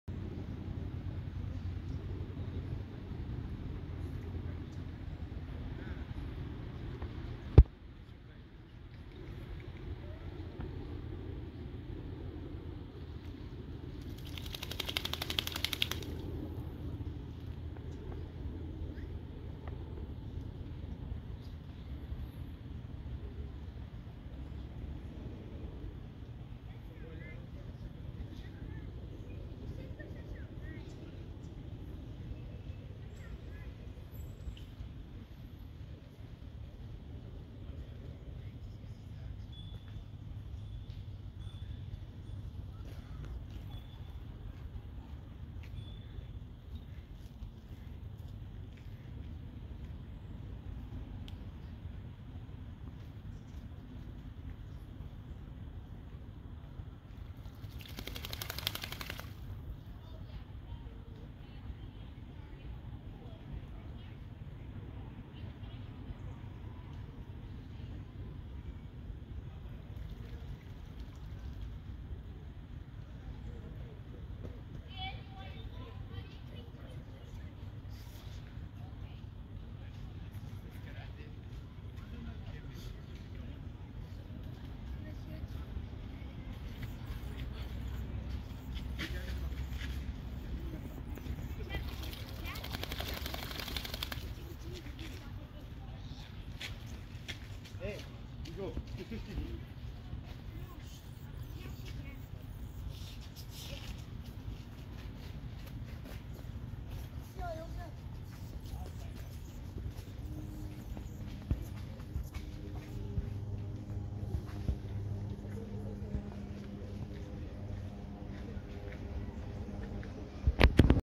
Video of duck (feat.3)
Incredible this time, at point not sure if is duck  or roaster wearing red rusty color mean brown or Dusk The Muscle Duck ...He really show is breast n pride too charming female ...